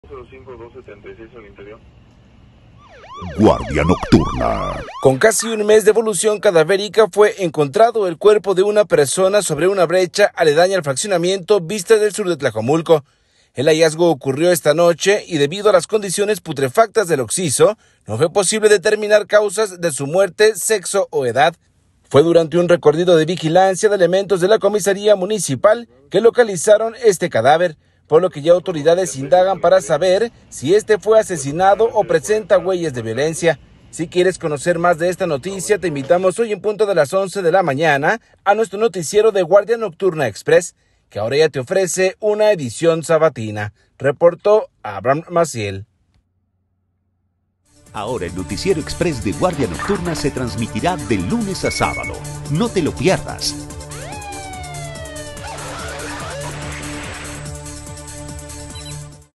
Previo Localizan cuerpo con un mes de evolución cadavérica
Preliminar Abandonan cuerpo encobija do y maniatado en Lomas de La Primavera, en Zapopan